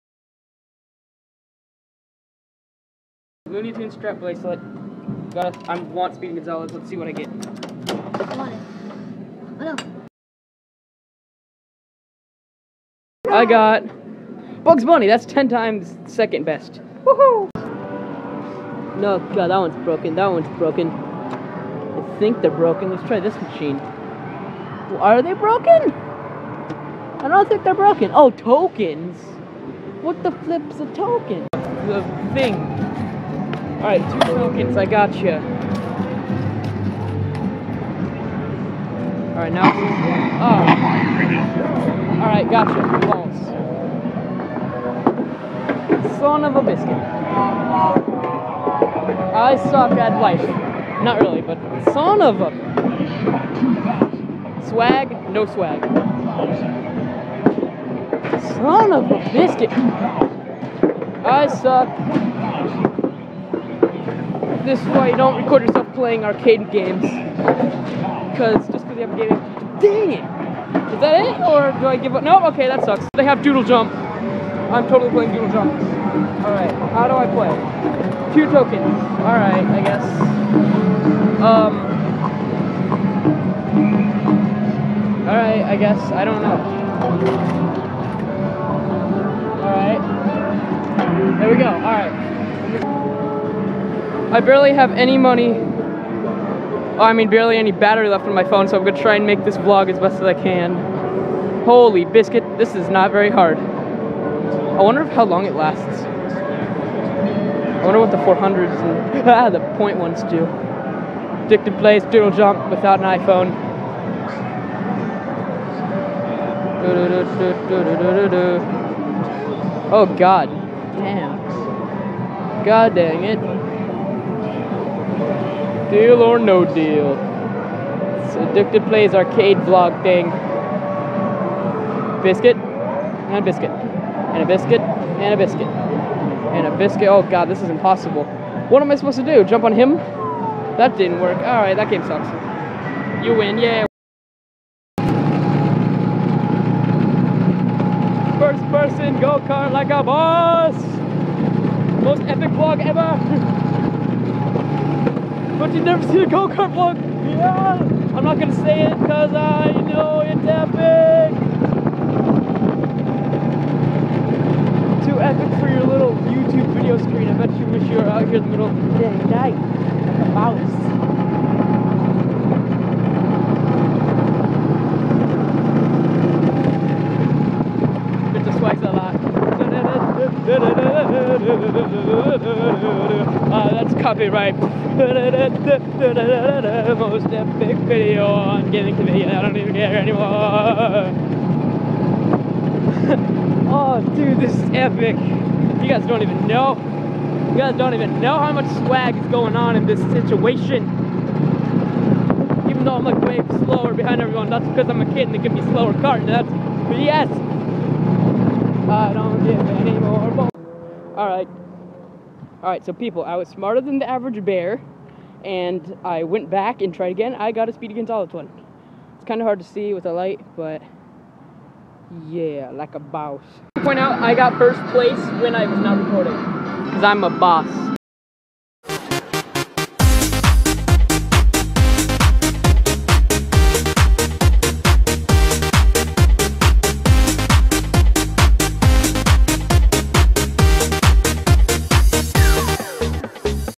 Dicted Vlogs | GO KART VLOG?
OMG I WENT ON A MARIO KART RIDE XD